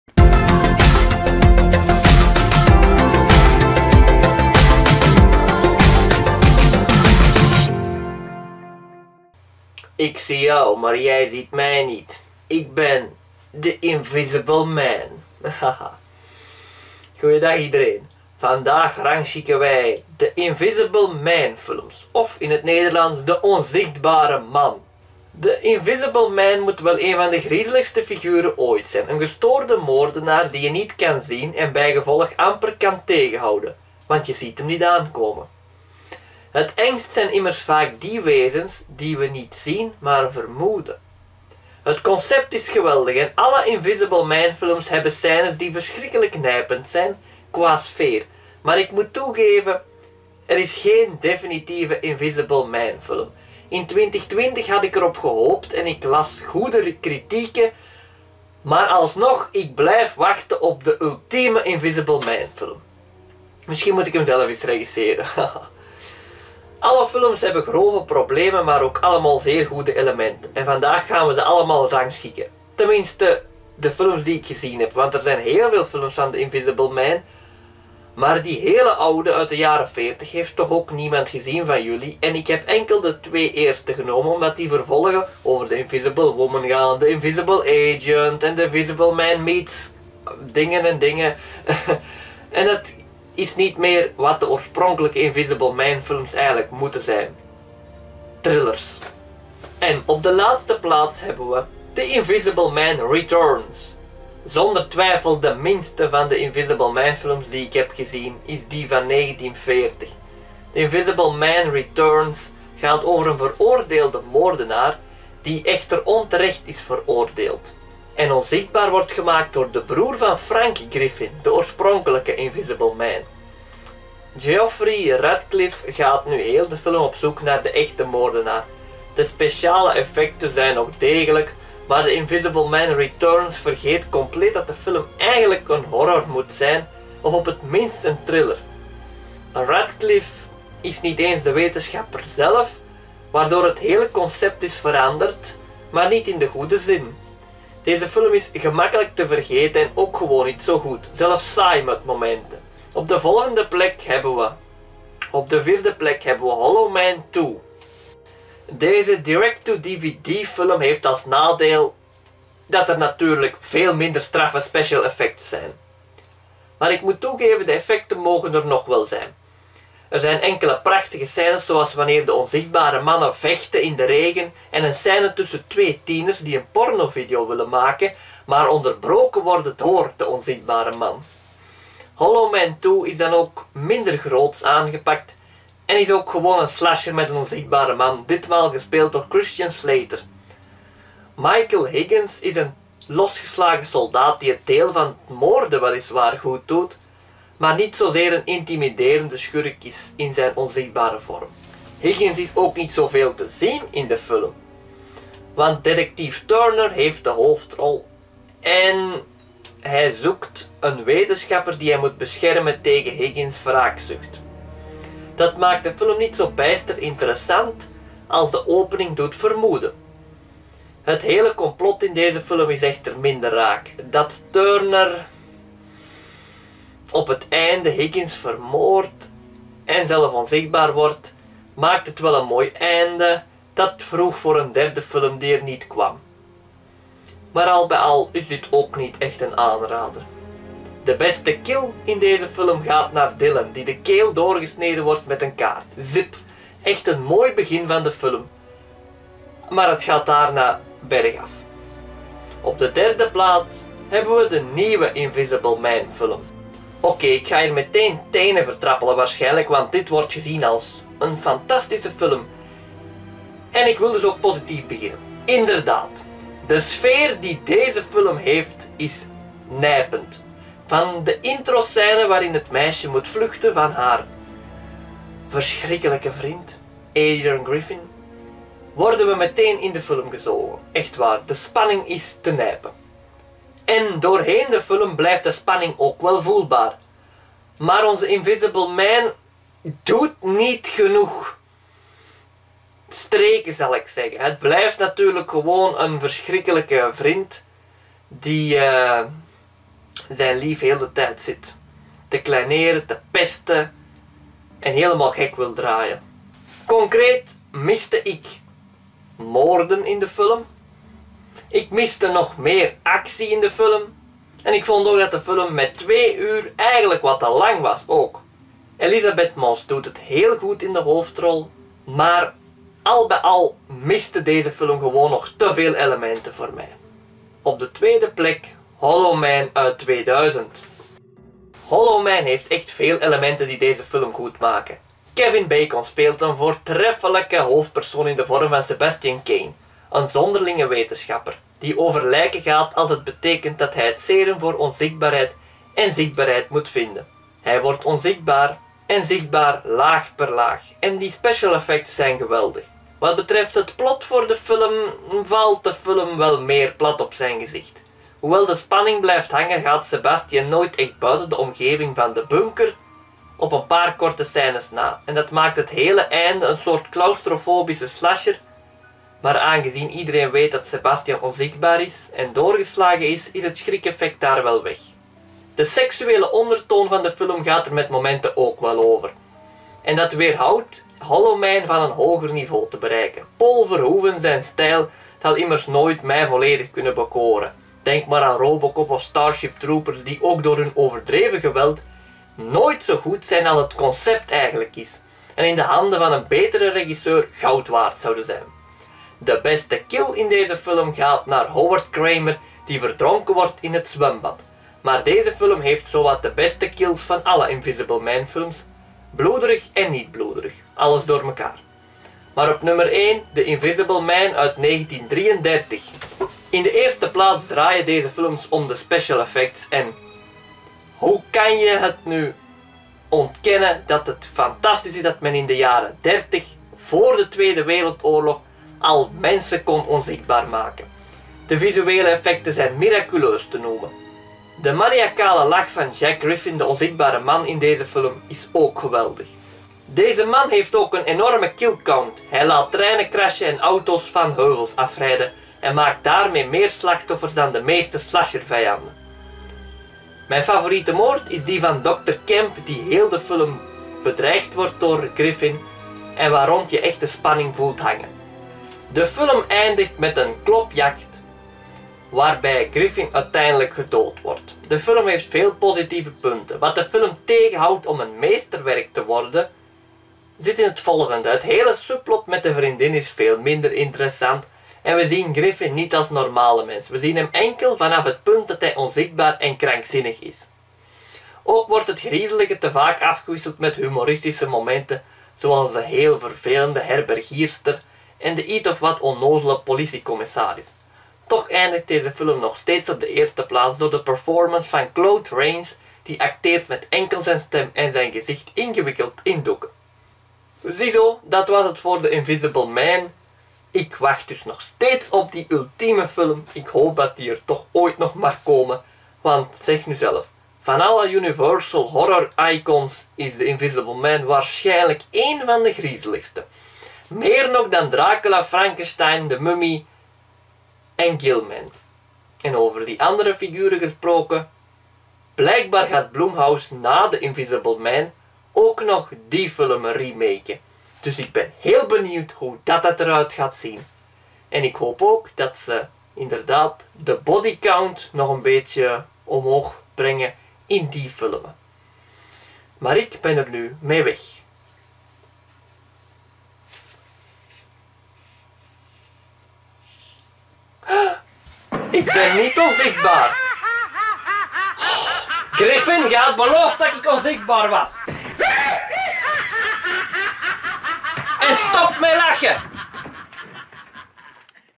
Ranking 5 Invisible Man films
The Invisible Man of de Onzichtbare Man is een klassiek horrorfiguur uit de jaren '30 dat nog steeds relevant kan zijn getuigd de nieuwe film uit 2020.
Ik kijk naar twee oude films, twee Hollow Man films én de nieuwe in deze ranking!